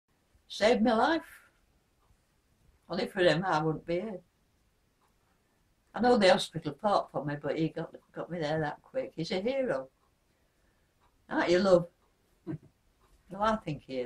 Lifesaving milkman
Iris Copeland on milkman Tony Stevens, who she says saved her life after she collapsed in the hallway of her home.